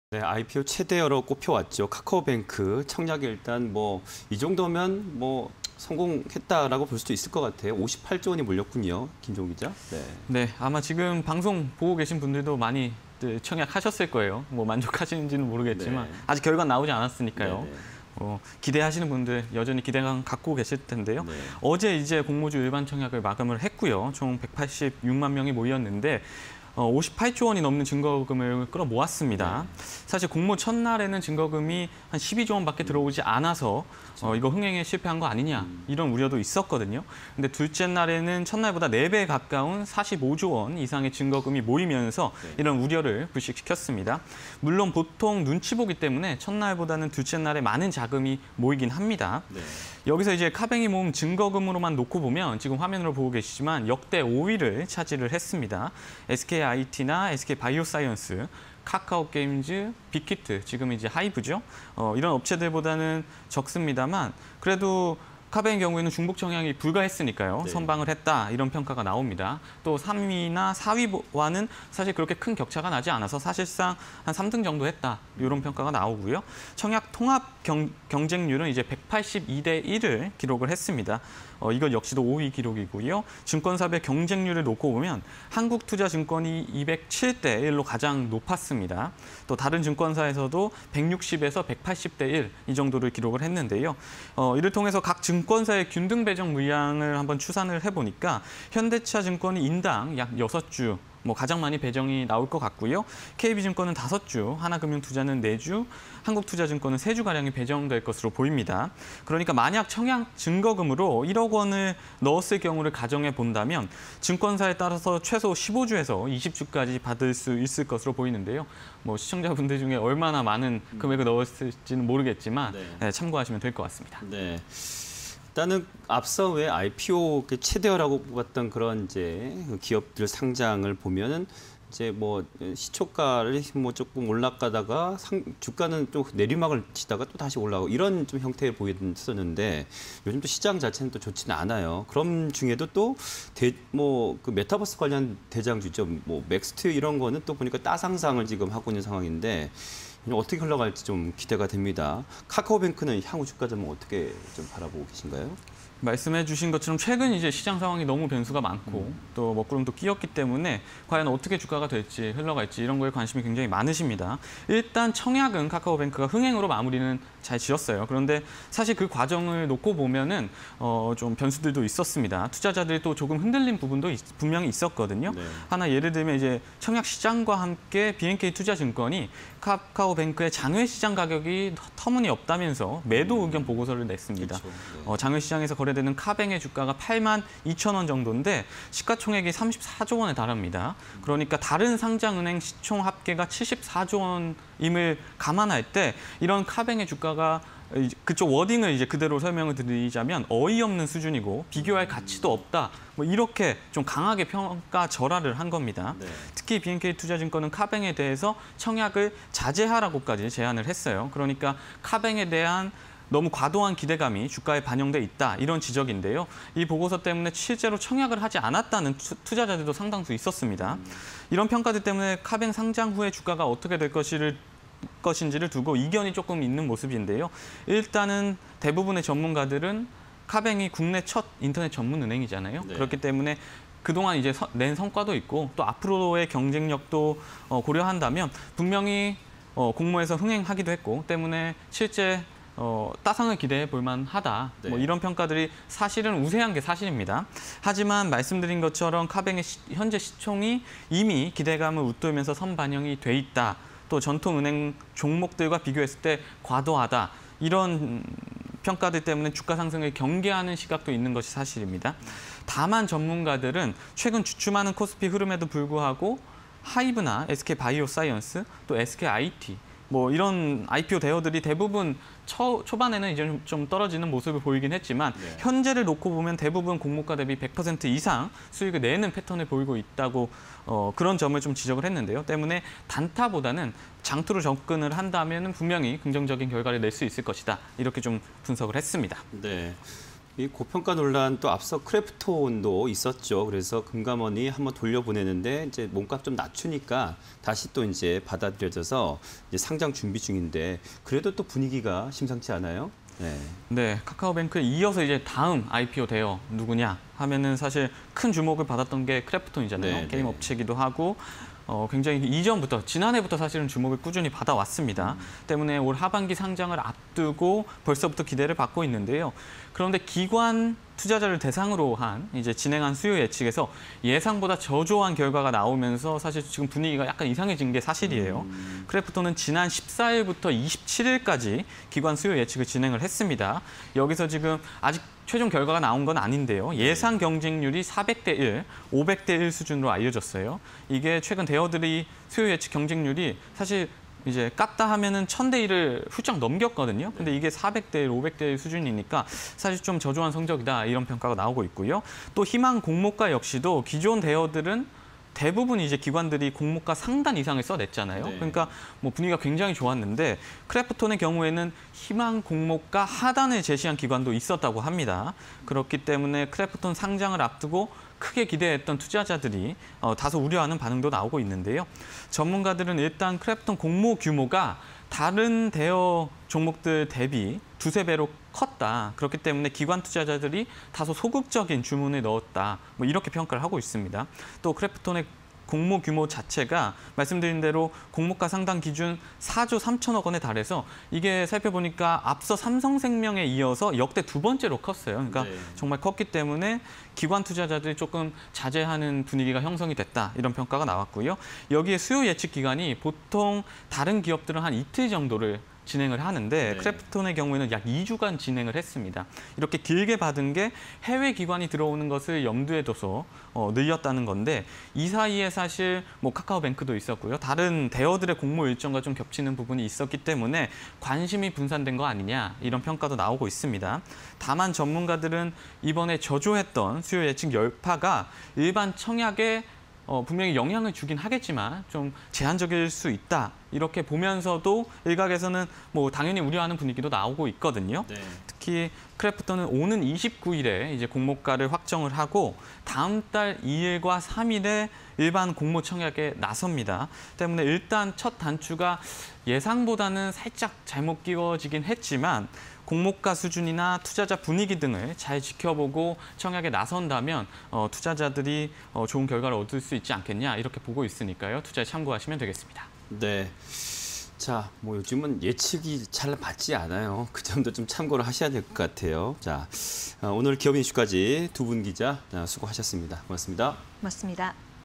카카오뱅크 청약에 58조 몰렸다... 몇주 받을까_기업IN이슈 (20210728)
- BNK투자증권, `매도 의견` 보고서... “주가 과도한 수준”
- “국내 첫 인터넷 전문은행으로 큰 성과... 따상도 기대”
- IPO 대어 주가, 공모가 웃돌아... “단타보다는 장투 접근”
- 기관 투자자 대상 수요예측서 예상보다 저조한 성적
- “공모 규모 너무 커 투자자들이 소극적... 관심도 분산”
- 오는 29일 공모가 확정... 다음달 2~3일 일반 공모 청약